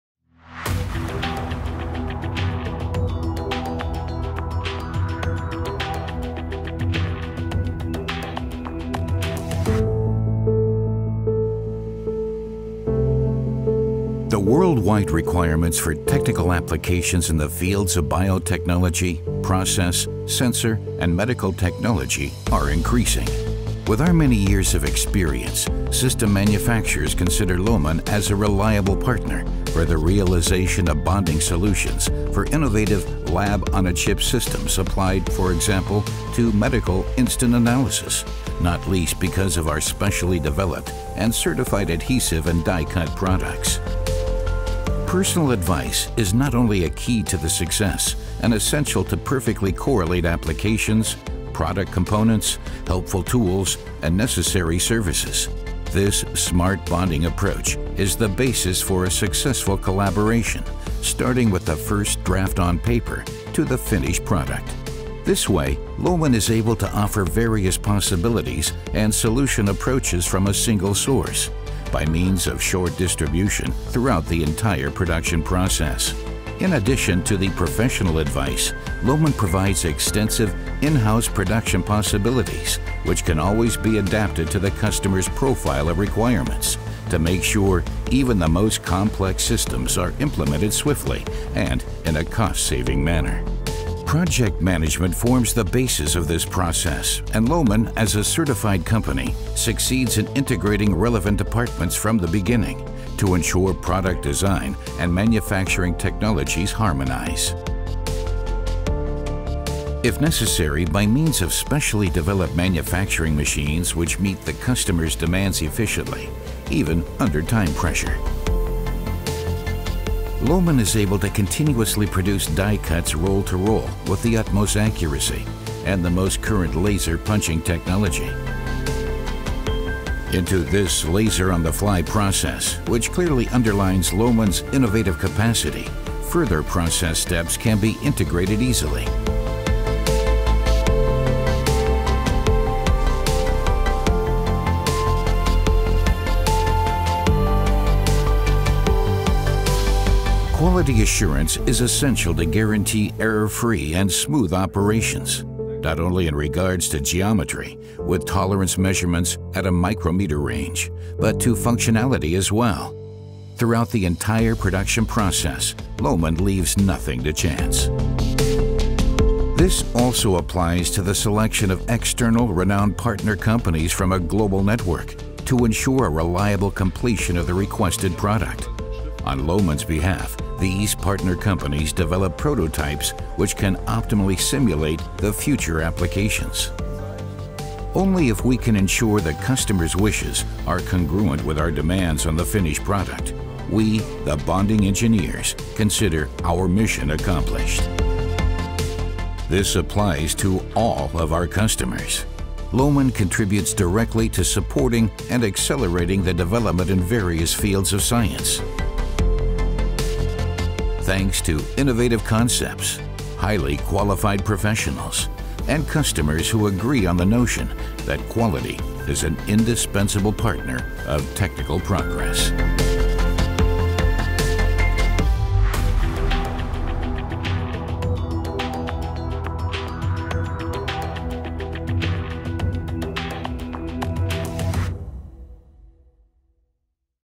Innovation „on-the-fly“ - Laser-die-cutting technology for highest demands
The Lohmann Competence Center Die-Cutting in Remscheid features years of experience in the realization of complex bonding solutions. The new “laser-on-the-fly process” is yet a new progress. Recently we accompanied this technology in the course an entire production process with the camera – from the initial, highly personalized consultation to the efficient implementation. Have a look for yourself!